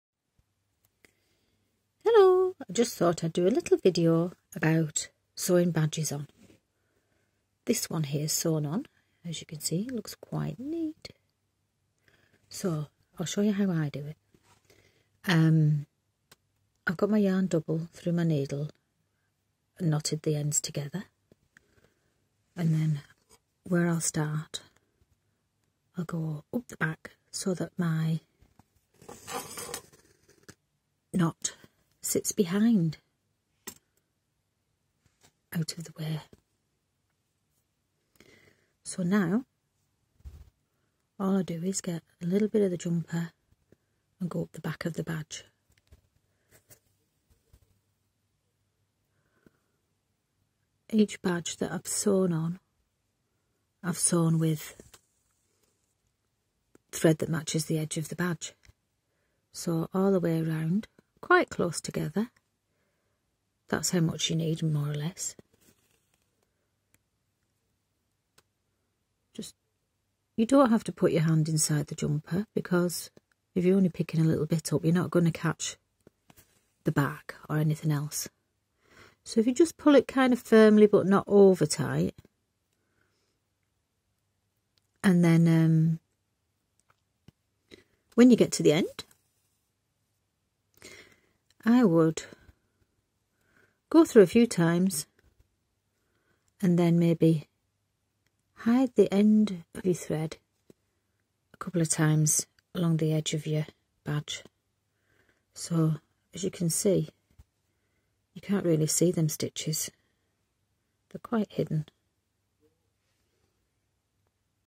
how to sew badges on uniforms
how to sew badges on to uniforms neatly by hand.
I use thread that matches the badge . thread your needle and away we go.